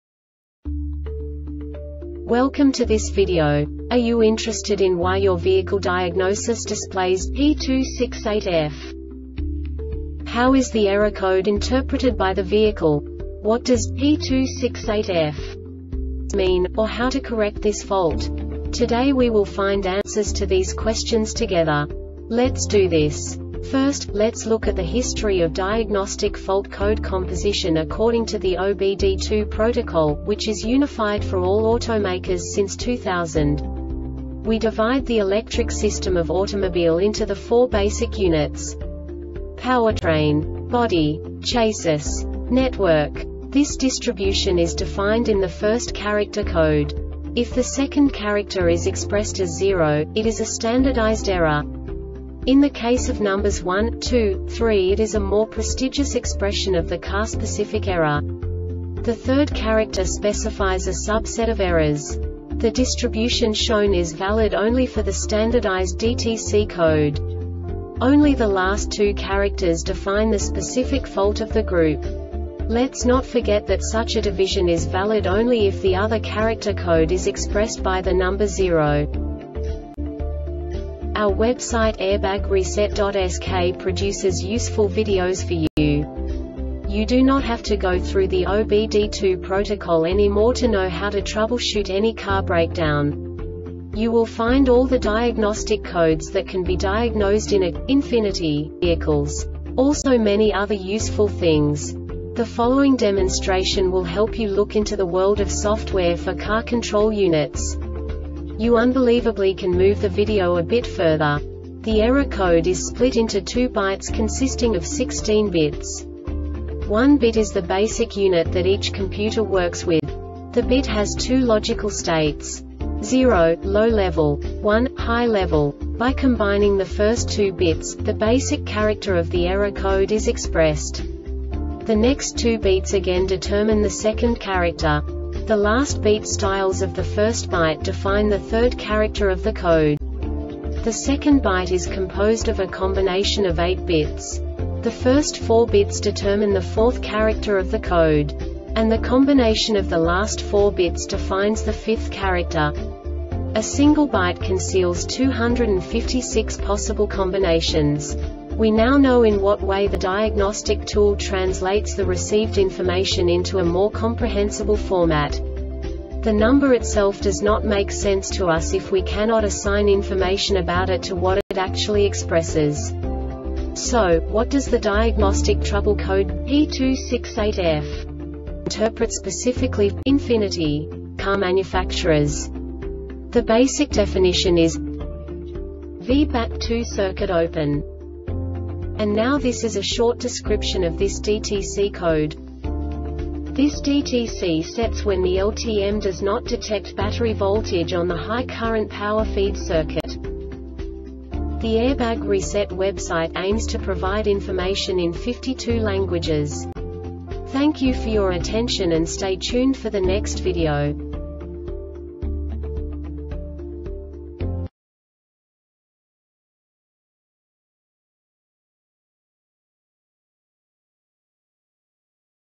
DTC Ford P268F Short Explanation
Contents:
0:21 Basic DTC analysis according to OBD2 protocol standard.
1:48 Insight into programming
2:58 A diagnostically understandable description of the Diagnostic Trouble Code
3:05 Basic error definition

Make:
Ford
Code:
P268F
Definition:
Vbatt.2 Circuit Open
Cause:
This DTC sets when the LTM does not detect battery voltage on the high current power feed circuit.